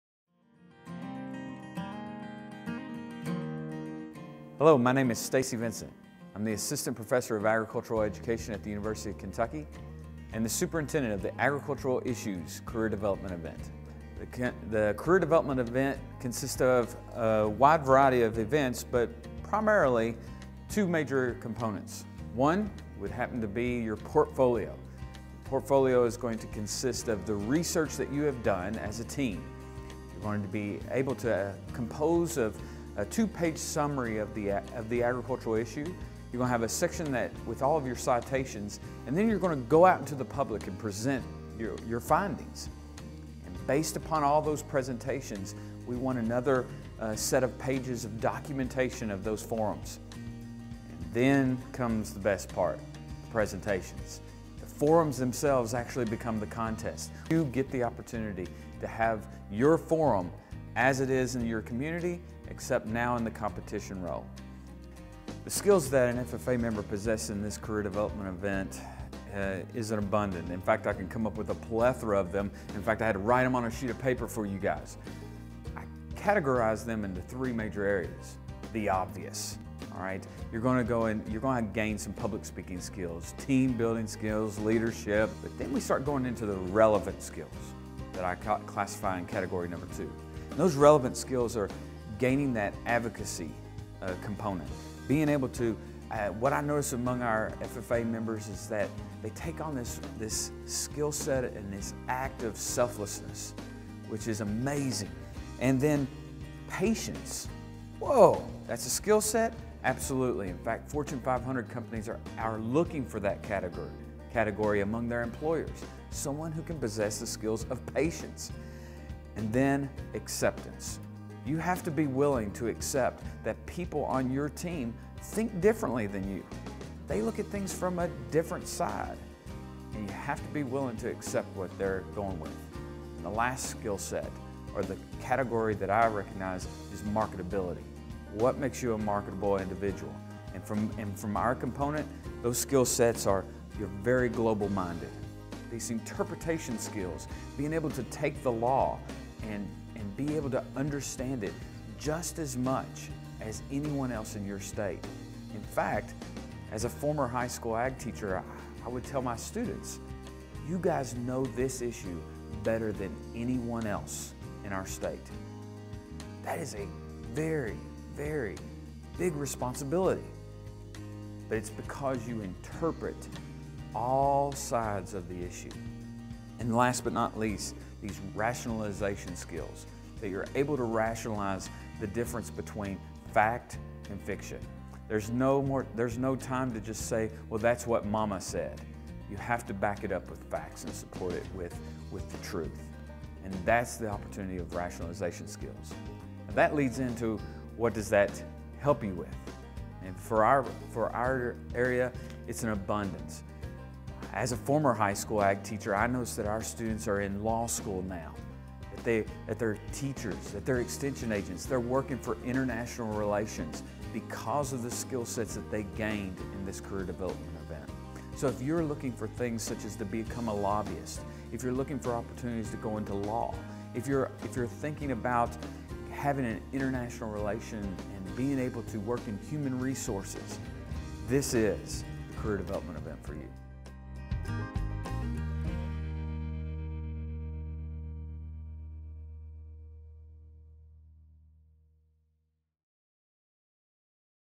Agricultural Issues Forum LDE
Participants research the pros and cons of an agricultural issue and present their findings to a panel of judges. @nationalffa1928